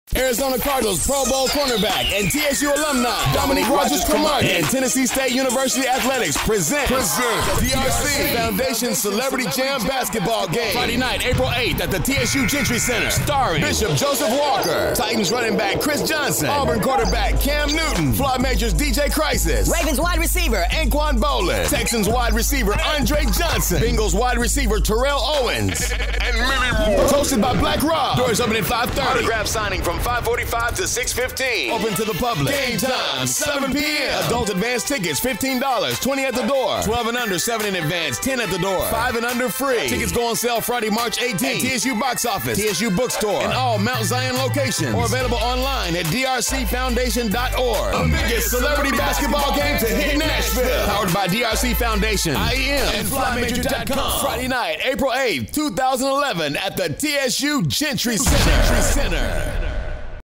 Dominque Rodgers Foundation Celebrity Slam Jam Basket Ball Game 2011 TSU
Dominque Rodgers Cormartie Foundation along with FlyMajor.com and Tennessee State University Athletic Department presents... The DRC Celebrity Basketball Slam Jam April 8, 2011 in the TSU Gentry Center. GAME TIME is 7PM

Game Celebrities include Bishop Joseph Walker, Chris Johnson, Cam Newton, DJ Crisis, Terrell "T.O." Owens, Andre Johnson, AnQuan Boldin ... And Many Many More!

Hosted By Black Rob

This commercial spot was brought to you in-part by McGee-Glover Media